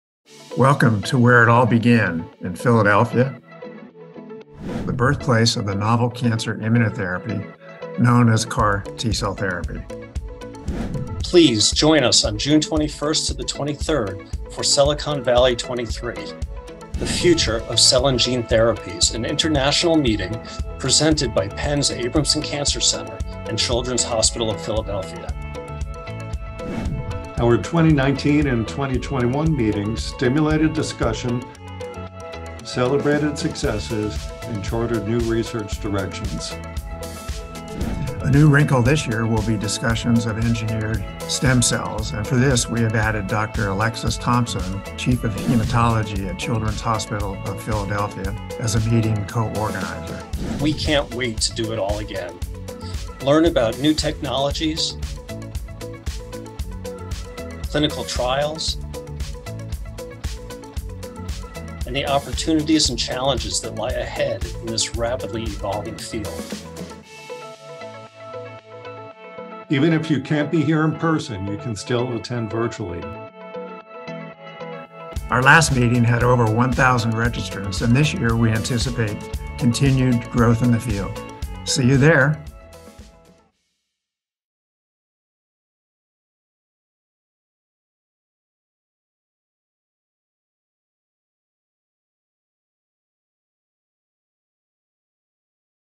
Join Us at Cellicon Valley '23: The Future of Cell and Gene Therapies
Welcome to the place where it all began – Philadelphia, the birthplace of the novel cancer immunotherapy known as CAR T-Cell Therapy. Please join us on June 21st to 23rd for Cellicon Valley ’23: The Future of Cell and Gene Therapies, an international meeting presented by Penn’s Abramson Cancer Center and Children’s Hospital of Philadelphia.